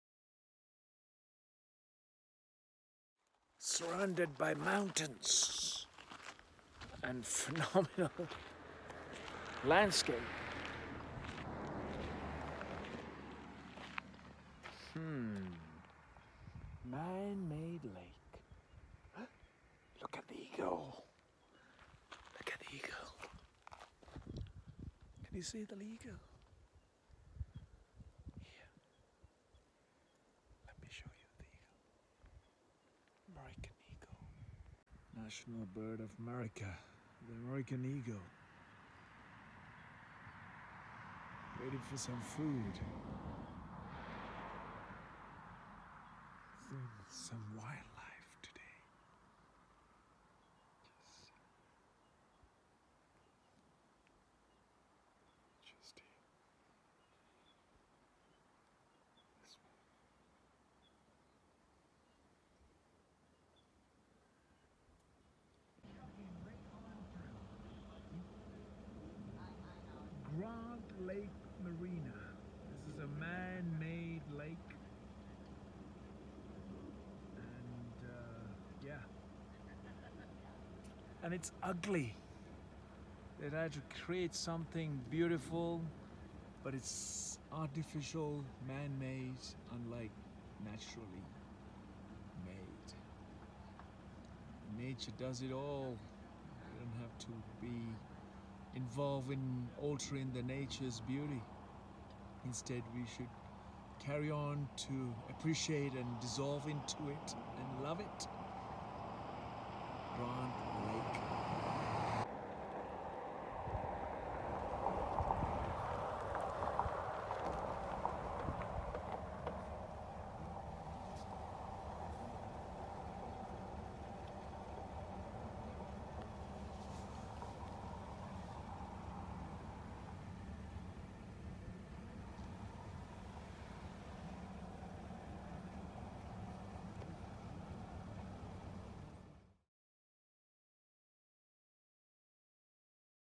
Live American Eagle Wild Life
Majestic fully alert American Eagle resting on a branch at grant lake.

I was joyful and excited to see a live and wild life.

Our wild and natural life is dying fast.

Natural habitat is being sacrificed to make away for our own comforts and conveniences.